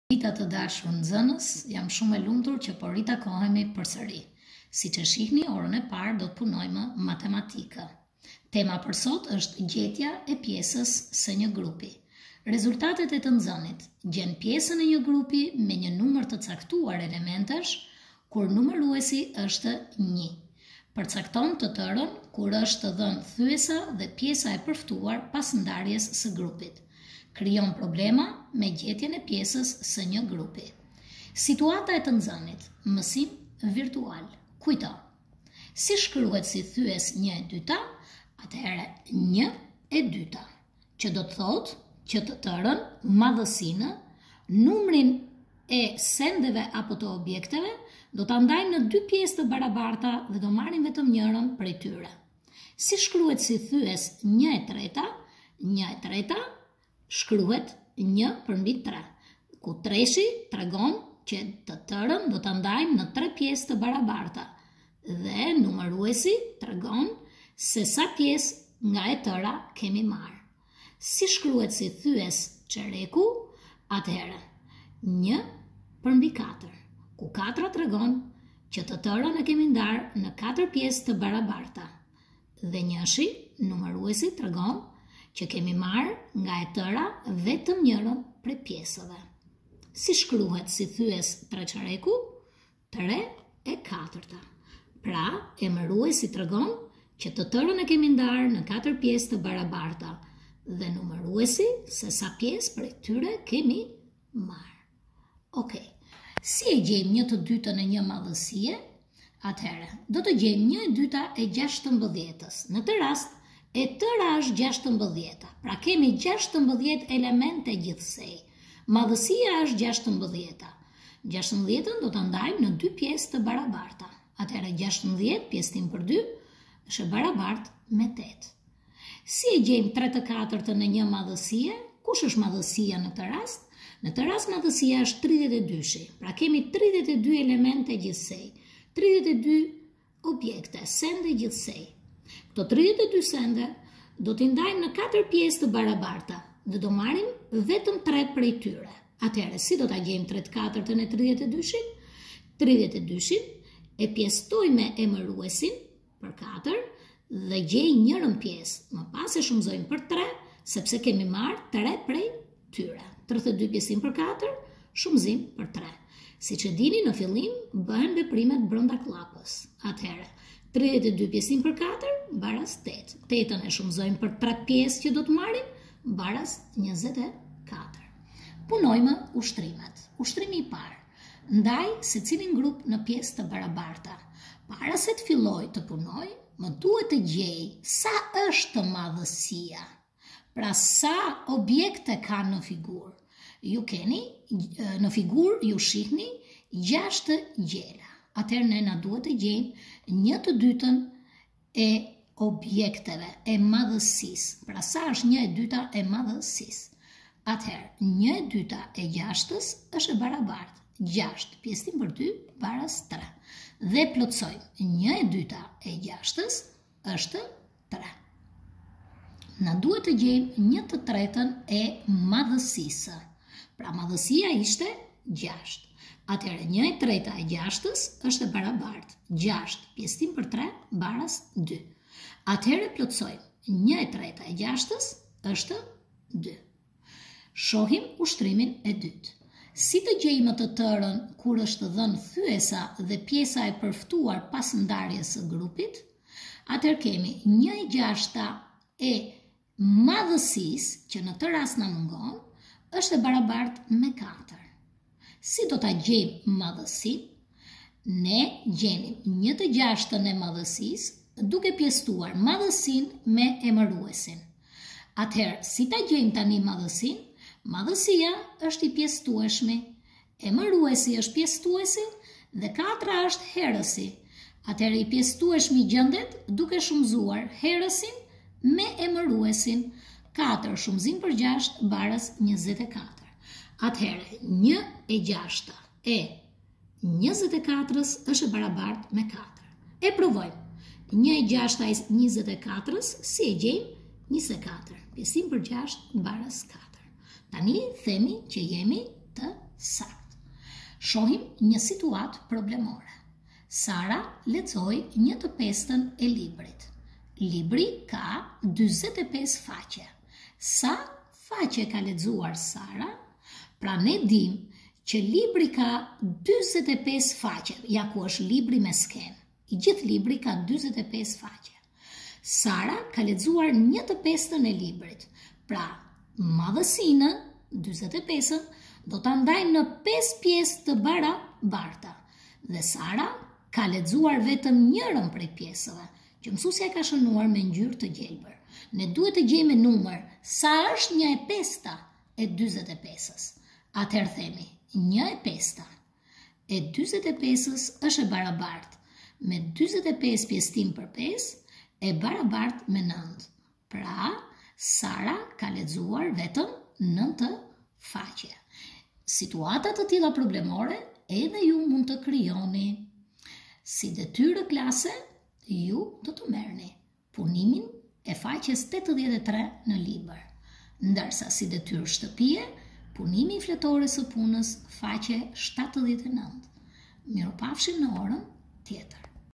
Shkolla "Bedrie Bebeziqi,,
Lenda :Matematike 3
Tema :Gjetja e pjeses se nje numri
Mesuese :Manuela Dobi